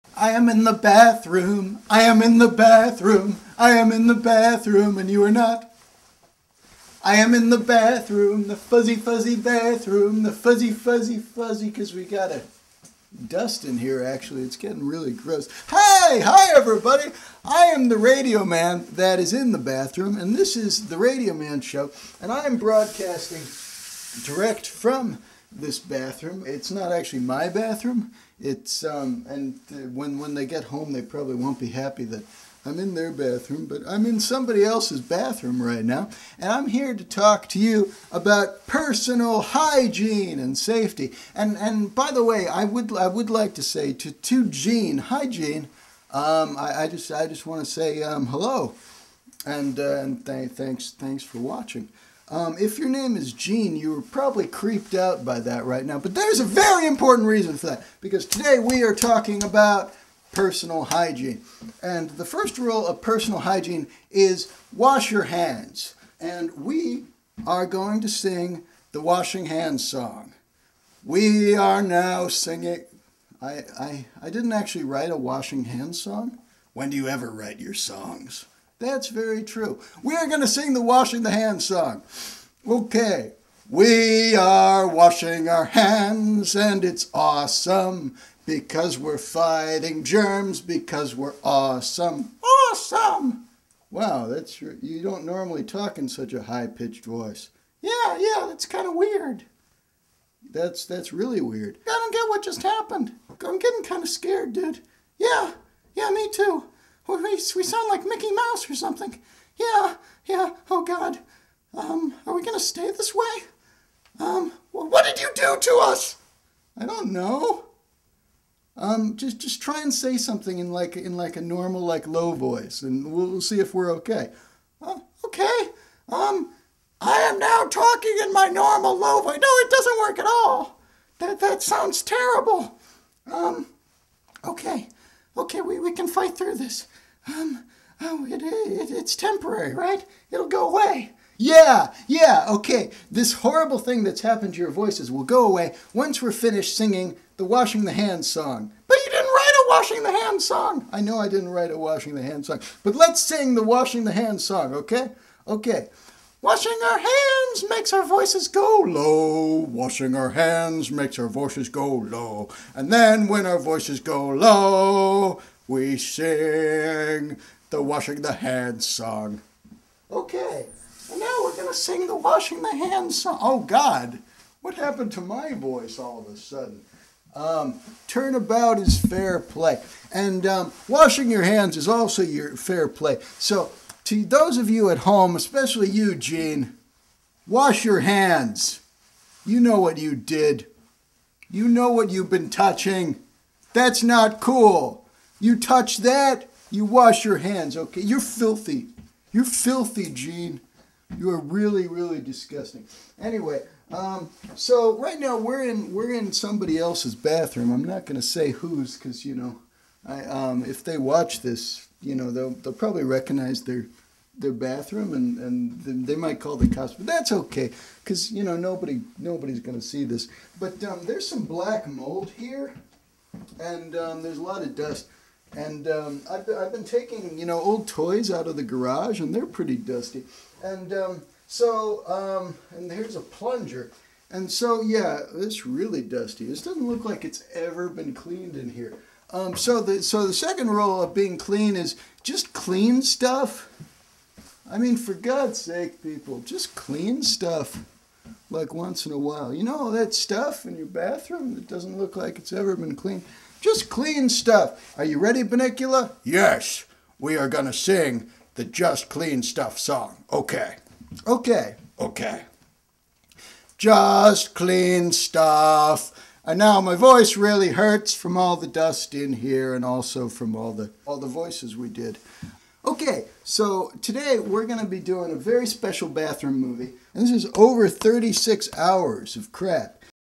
Radio Man in the Bathroom / Hygiene Special
Radio Man is in the bathroom, teaching you how to take better care of your hygiene, with the help of Bunnicula and the CultureDogs.